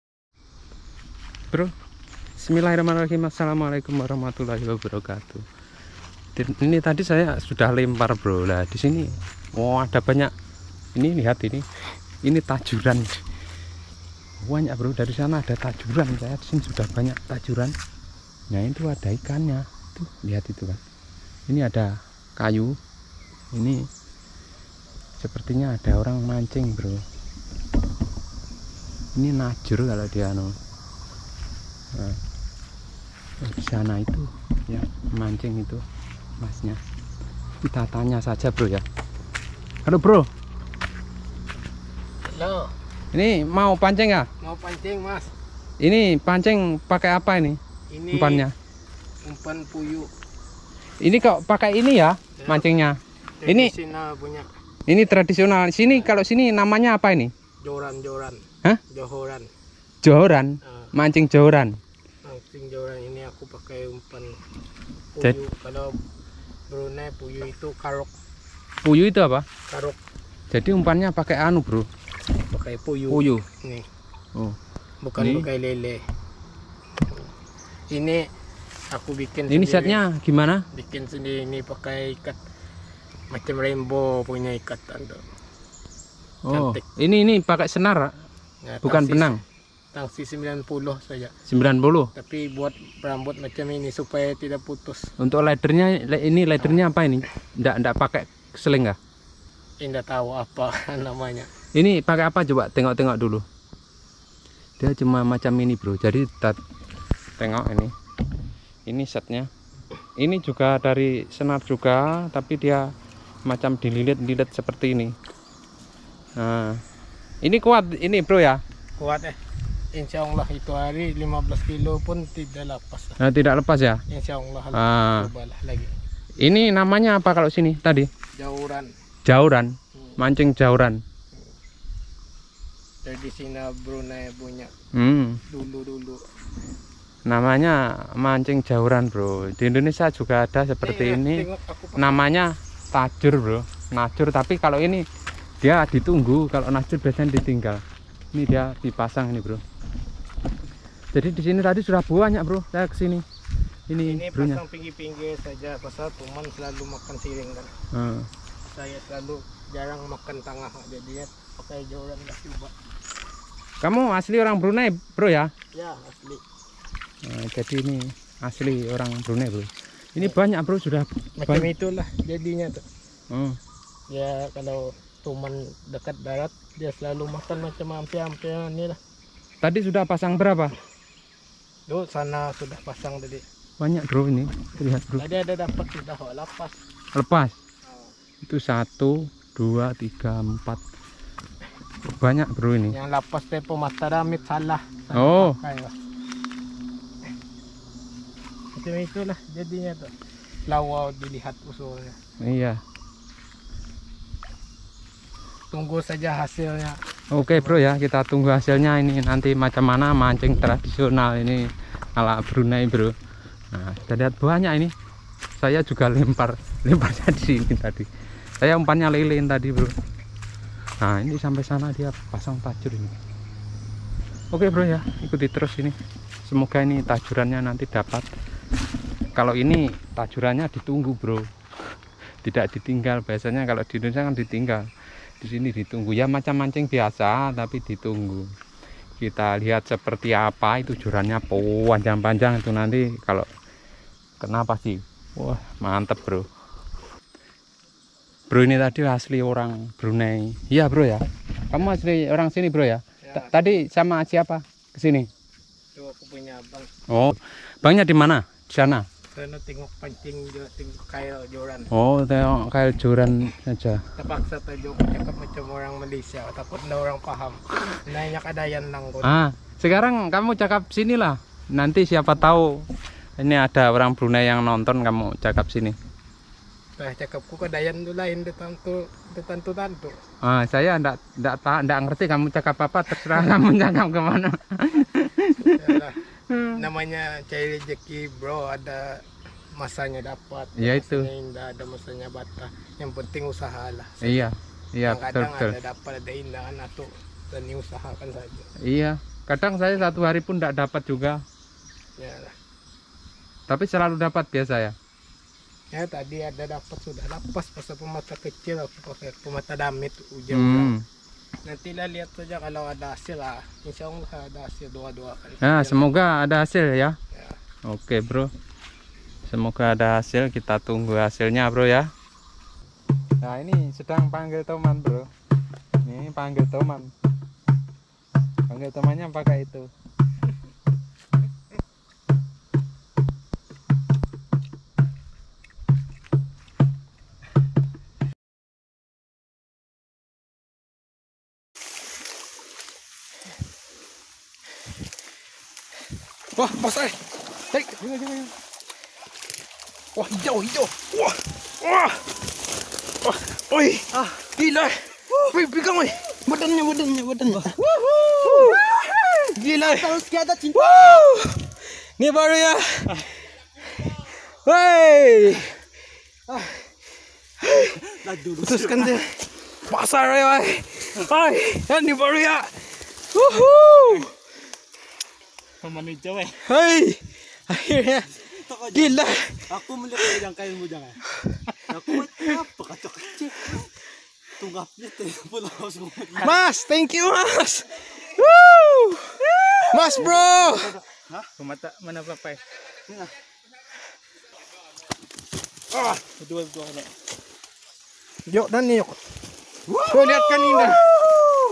TRADITIONAL SNAKEHEAD FISHING..
Najur ikan toman umpan papuyu/ betok